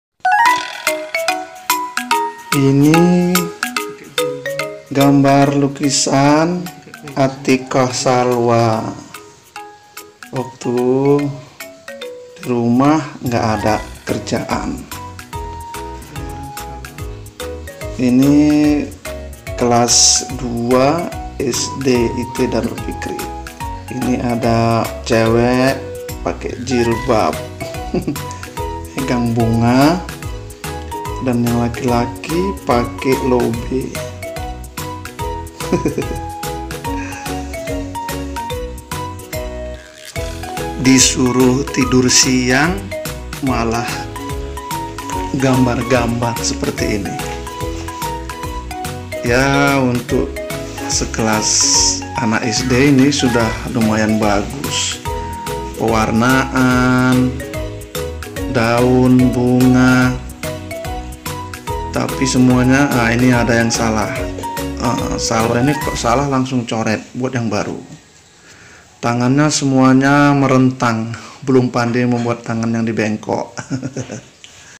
LUCU !! Kreasi Atiqah Salwa Menggambar Bebas 😂 😁
LUCU !! Kreasi Atiqah Salwa Panjaitan Menggambar Bebas anak laki laki muslim dengan pakai lobe dan anak perempuan dengan jilbabnya dan memegang bunga..
note: atiqah saat kelas 2 sekolah dasar.